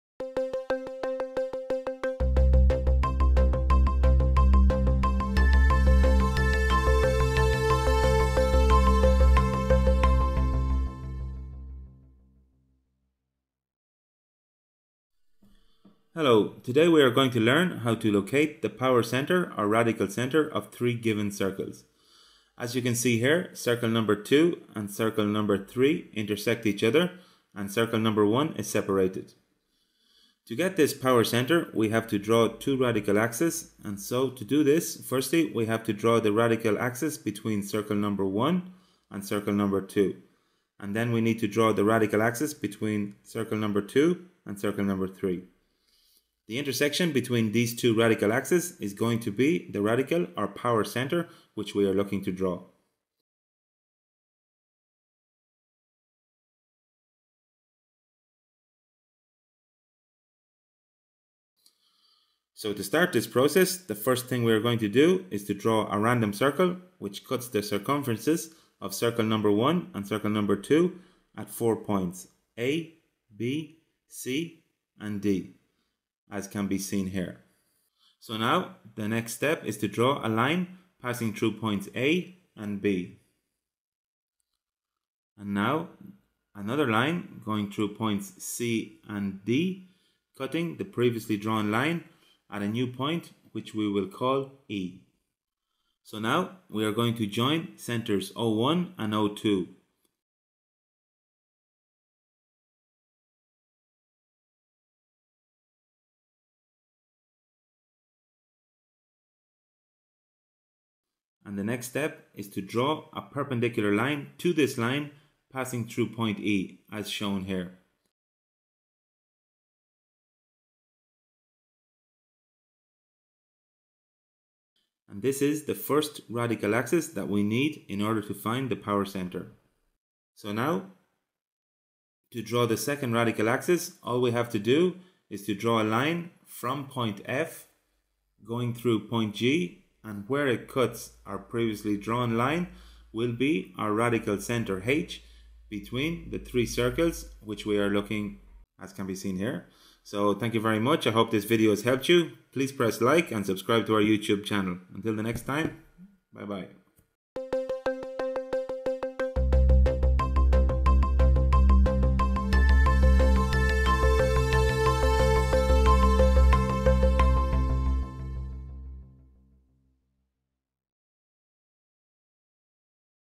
How to find the Power Center of three circles (Radical Center)
Learn how to locate the Radical center of 3 circumferences. The radical center is the intersection point of the three radical axes of the pairs of circles.